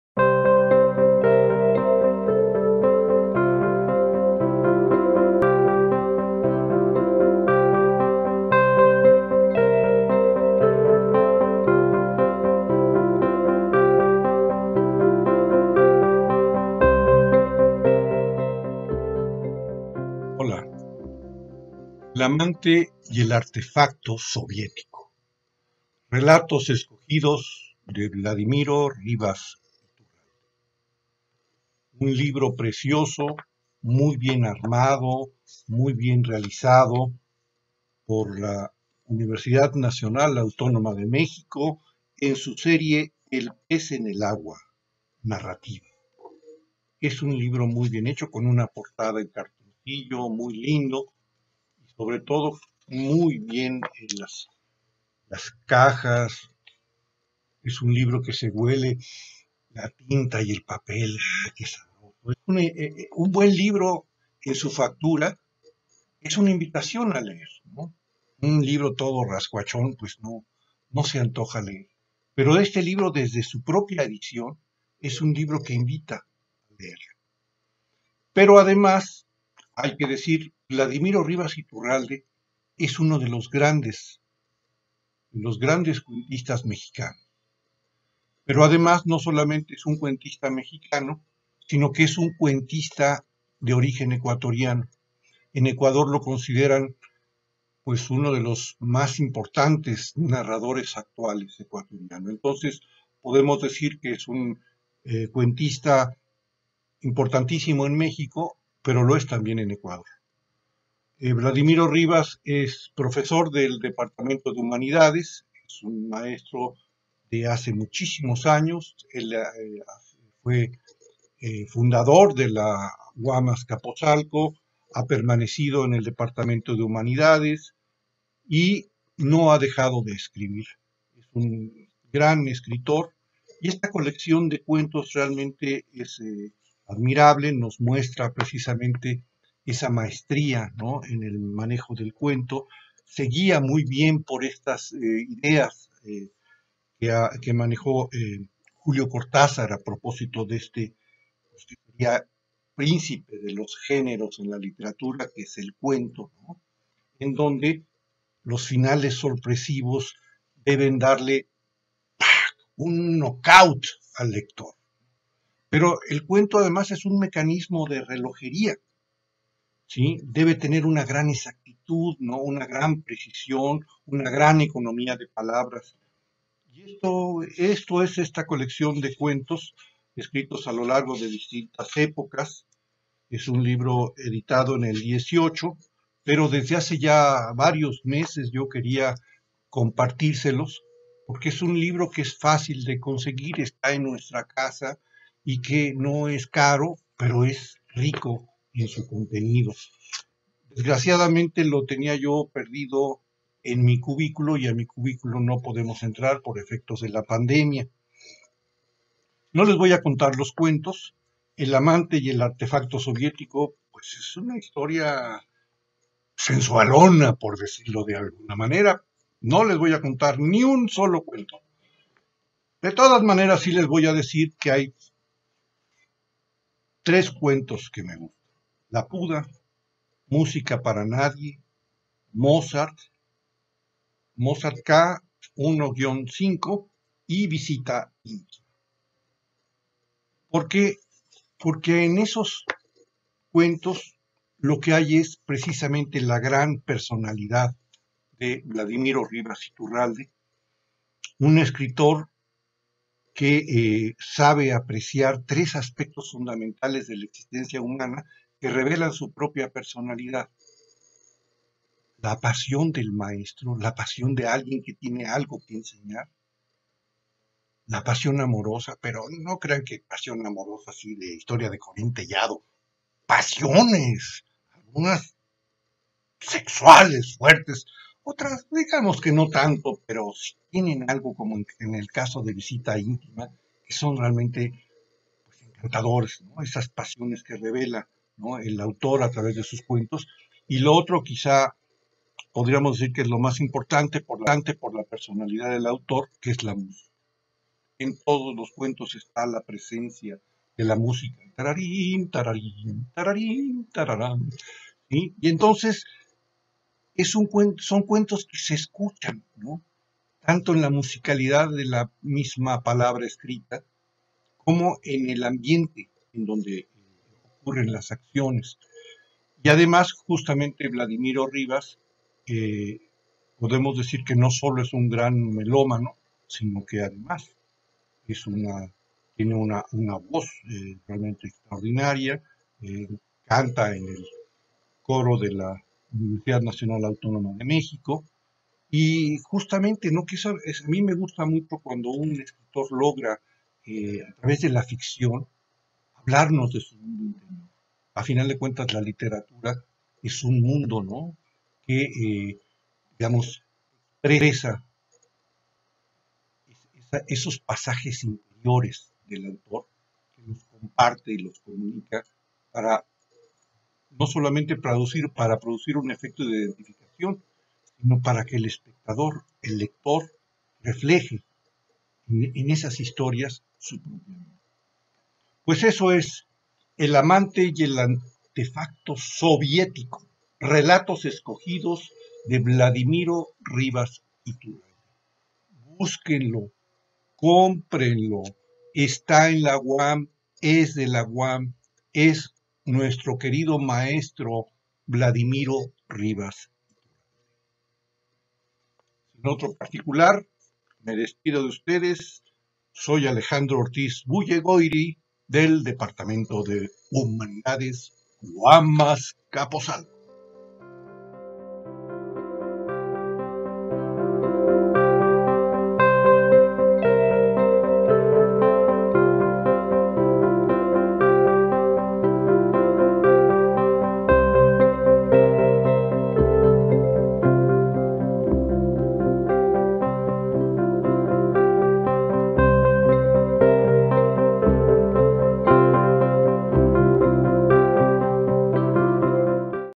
LEER es... dialogar en silencio. Buenas recomendaciones de buenas lecturas parte 27
🎥Te presentamos el video de esta semana de la serie: "LEER es... dialogar en silencio. Buenas recomendaciones de buenas lecturas."
Para disfrutar de lecturas recomendables. 🤓

📕El amante y el artefacto soviético. Autor: Vladimiro Rivas.

🗣 Comentada por: Alejandro Ortiz. UAM-A.